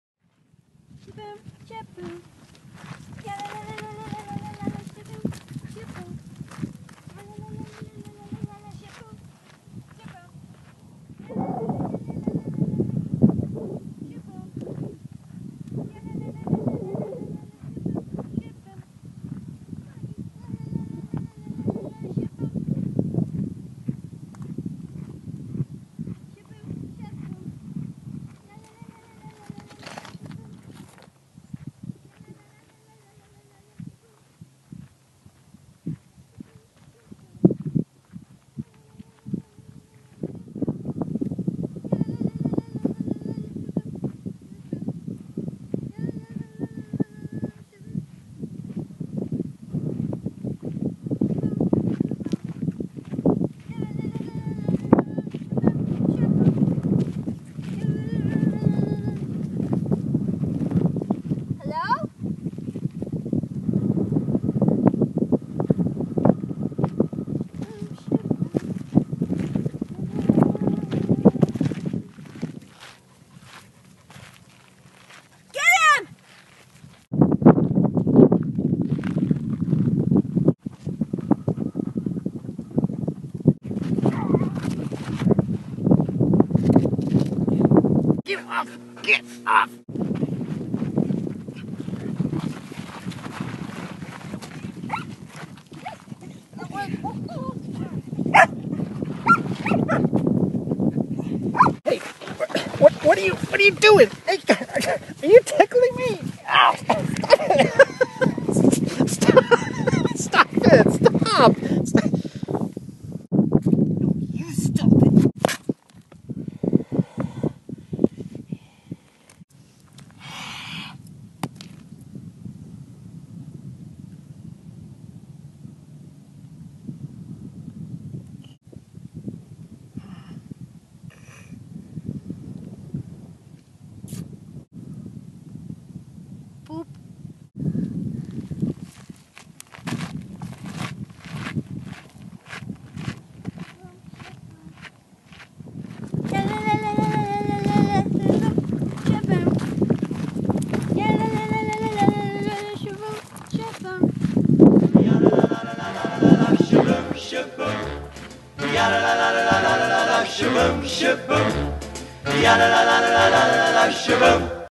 Desert Predator
A short horror movie starring Marty Beckers, Wendy Beckers, Pickles, and Licky!  Shot in the desert, entirely with an iPad, just for the heck of it.